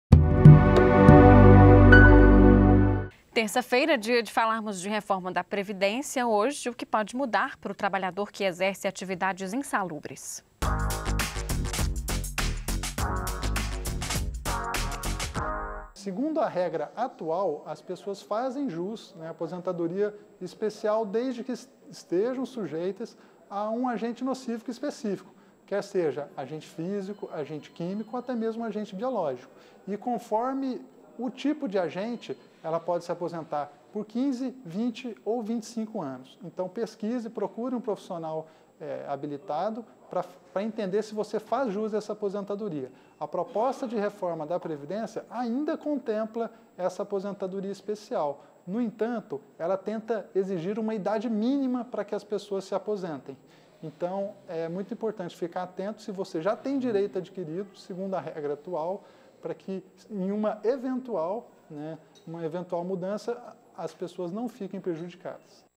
REFORMA DA PREVIDÊNCIA: TRABALHADORES EM ATIVIDADES INSALUBRES - Jornal Minas
Para continuar a série sobre a Reforma da Previdência, vamos abordar o que pode mudar, na nova proposta, para os trabalhadores que exercem atividades insalubres. Confira!